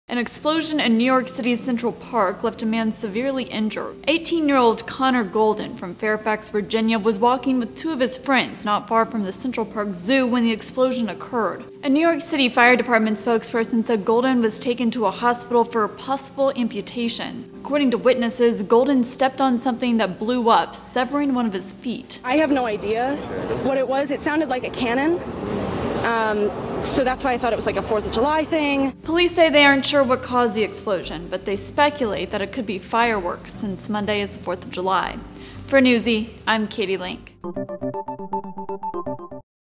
An Explosion In Central Park Severed A Man's Foot
An explosion in New York City's Central Park left a man severely injured.

Eighteen-year-old Connor Golden from Fairfax, Virginia, was walking with two of his friends not far from the Central Park Zoo when the explosion occurred.

A New York City Fire Department spokesperson said Golden was taken to a hospital for "possible amputation."

According to witnesses, Golden stepped on something that blew up, severing one of his feet.

"I have no idea what it was. It sounded like a cannon. So that's why I thought it was like a Fourth of July thing," one witness told CNN.

Police say they aren't sure what caused the explosion, but they speculate that it could be fireworks since Monday is the Fourth of July.

This video includes images from Getty Images and clips from NY1, WABC and CNN.